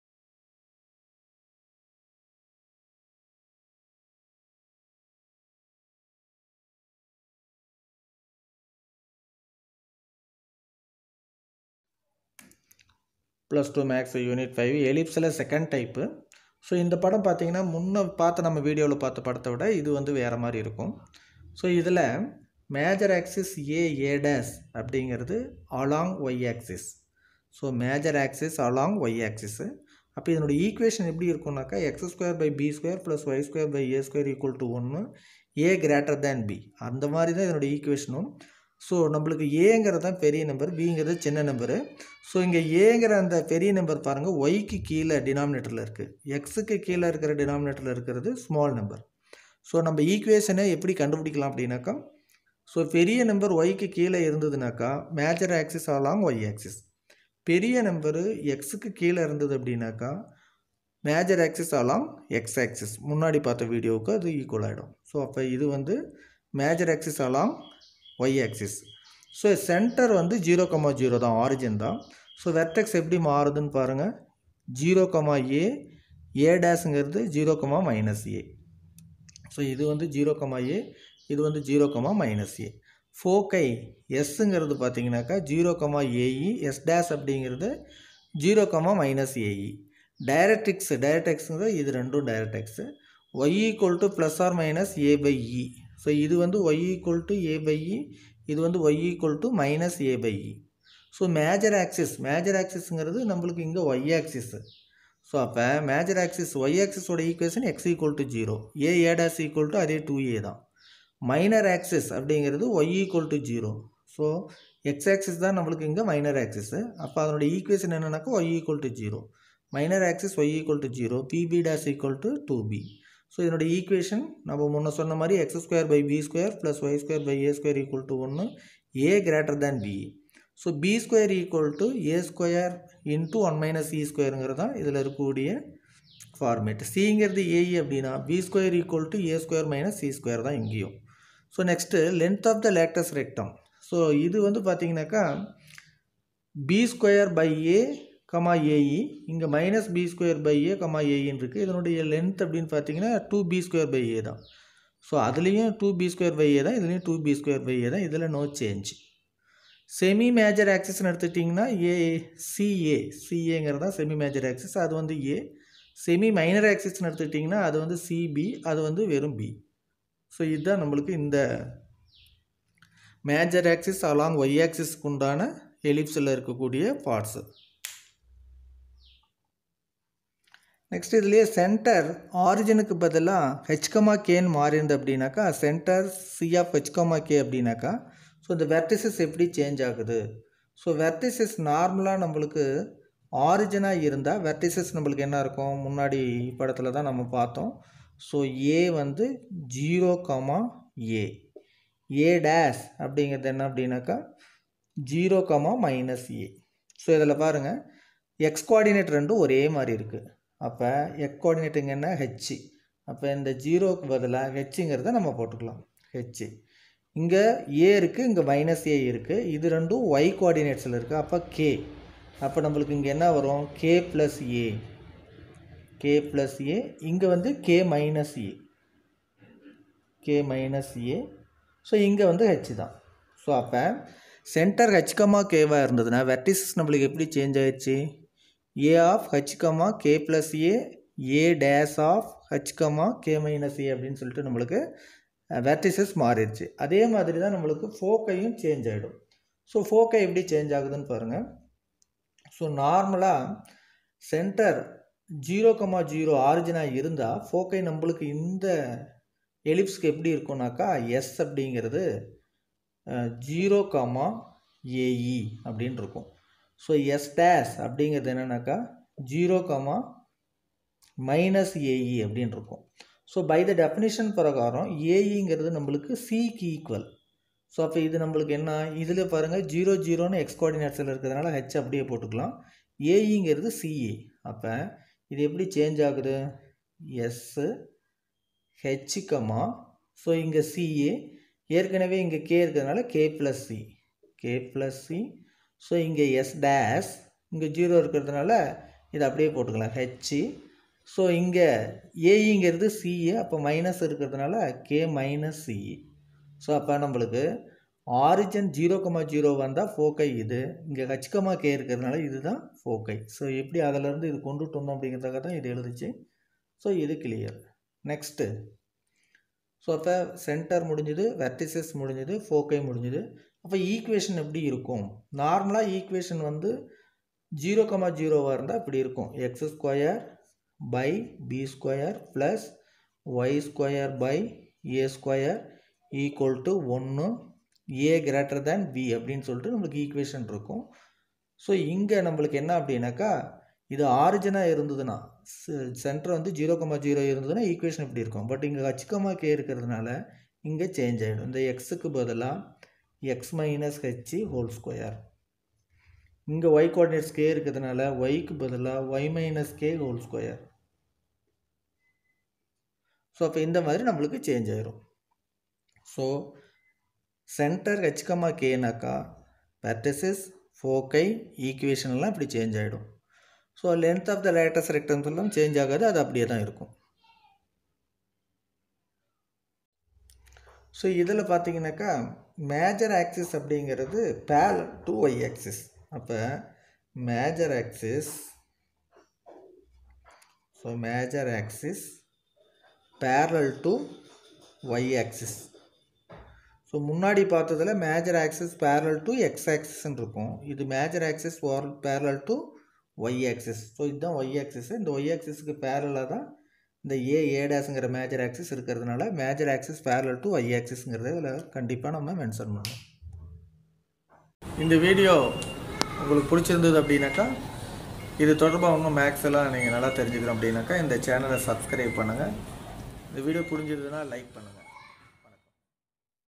12th Maths volume 1 Unit 5
Definition of Ellipse Major axis parallel to y-axis and centre(h, k)